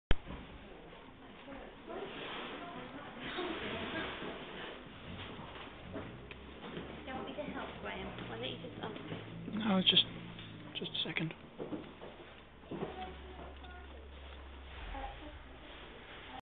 bearlee walking
wooden teddy bear out for a walk. hand operated automaton, walks on the spot whilst his handle is turned.